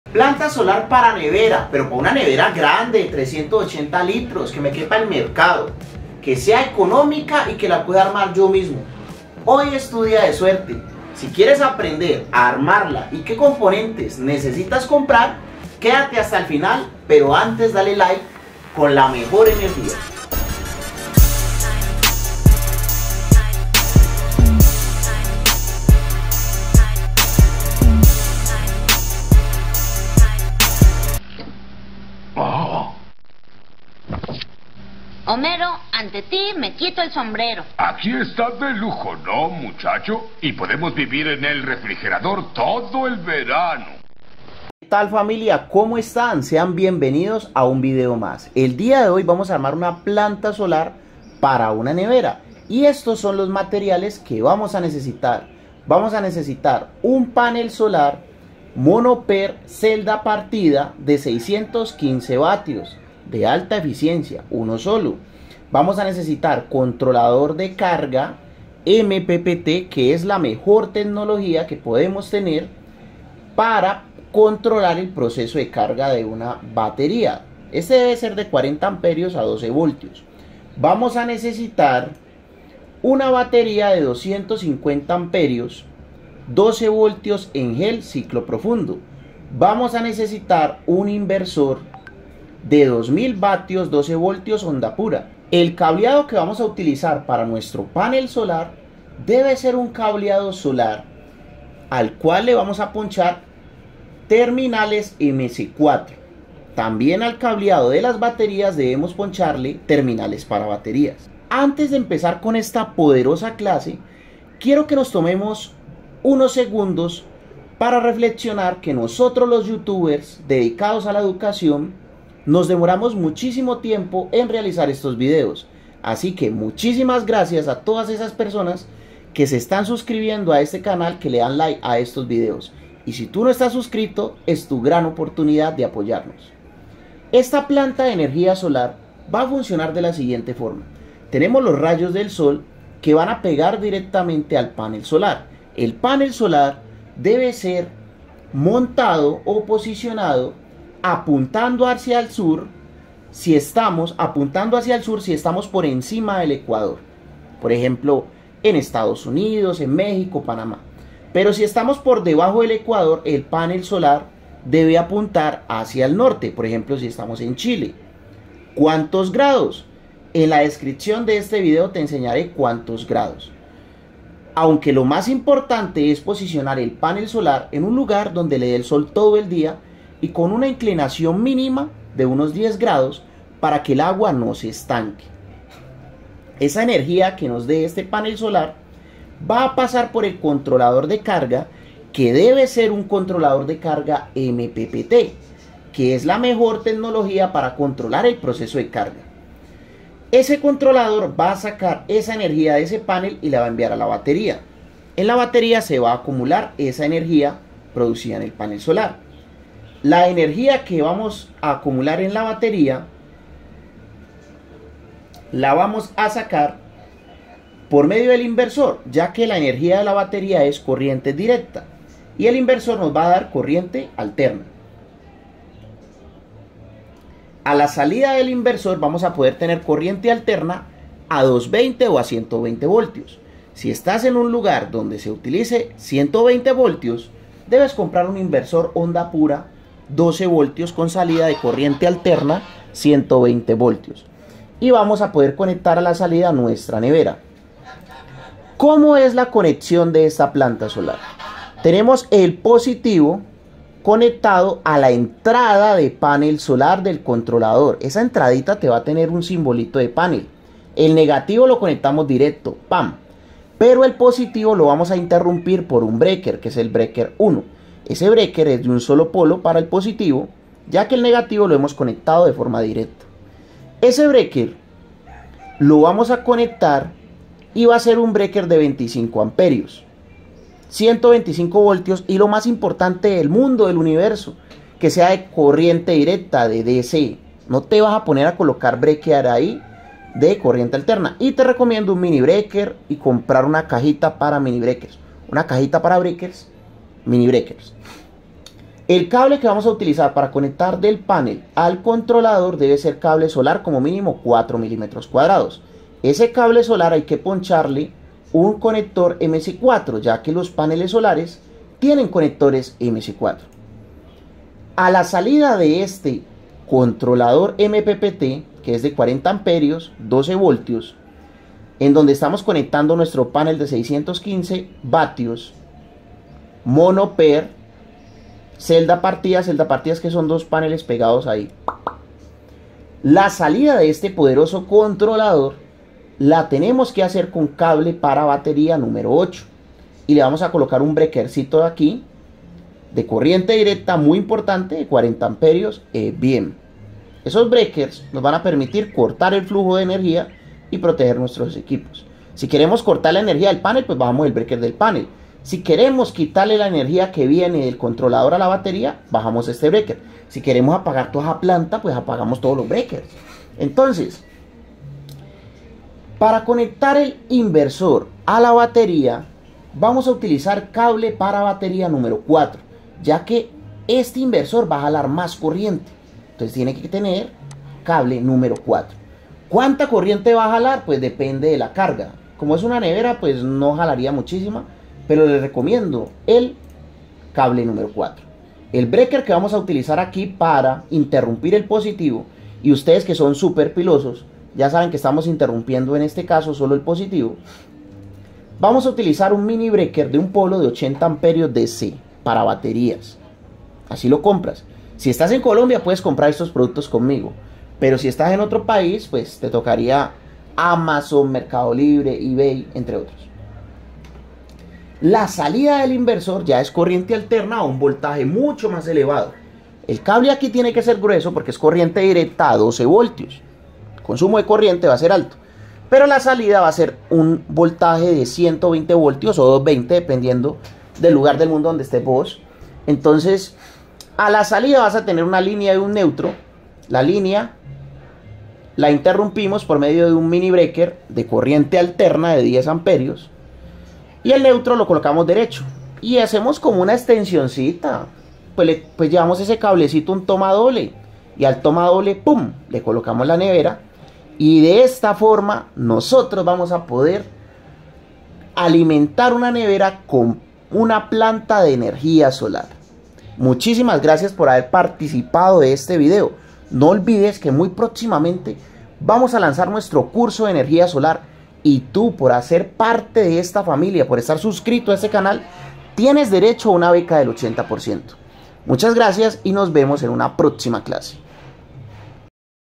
✅Nevera con panel solar. Armarla vos mismo #2023
Arma vos mismo un panel solar para una nevera. cuantos paneles solares necesita una nevera
Es hora de hacerlo

participa de nuestro próximo curso con tu beca del 80%🧑‍🏫🧑‍🏫🧑‍🏫

venta de productos solo en Colombia
asesorías a nivel mundial cotiza la tuya